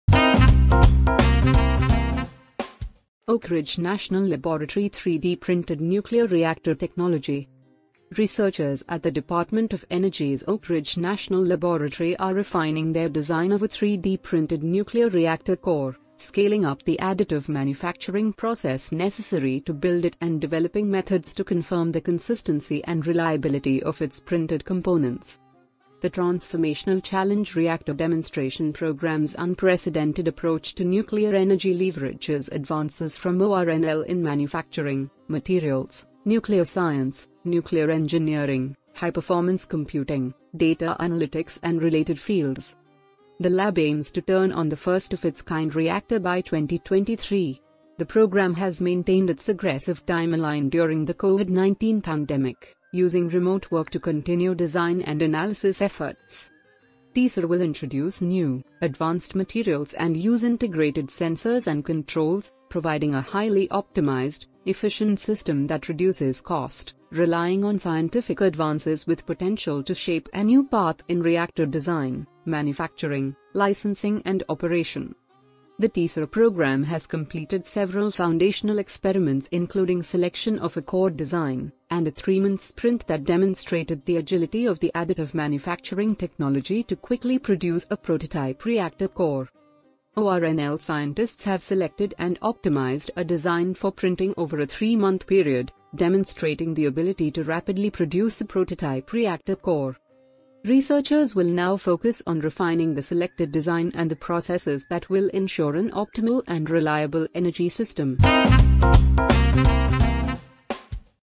Oak Ridge National Laboratory 3D Printed Nuclear Reactor Technology
Researchers at the Department of Energy’s Oak Ridge National Laboratory are refining their design of a 3D-printed nuclear reactor core, scaling up the additive manufacturing process necessary to build it and developing methods to confirm the consistency and reliability of its printed components.

The Transformational Challenge Reactor Demonstration Program’s unprecedented approach to nuclear energy leverages advances from ORNL in manufacturing, materials, nuclear science, nuclear engineering, high-performance computing, data analytics and related fields.

The lab aims to turn on the first-of-its-kind reactor by 2023. The program has maintained its aggressive timeline during the COVID-19 pandemic, using remote work to continue design and analysis efforts.

TCR will introduce new, advanced materials and use integrated sensors and controls, providing a highly optimized, efficient system that reduces cost, relying on scientific advances with potential to shape a new path in reactor design, manufacturing, licensing and operation.

The TCR program has completed several foundational experiments including selection of a core design, and a three-month “sprint” that demonstrated the agility of the additive manufacturing technology to quickly produce a prototype reactor core.

ORNL scientists have selected and optimized a design for printing over a three month period, demonstrating the ability to rapidly produce a prototype reactor core.

Researchers will now focus on refining the selected design and the processes that will ensure an optimal and reliable energy system.

Strategic Research Institute, SteelGuru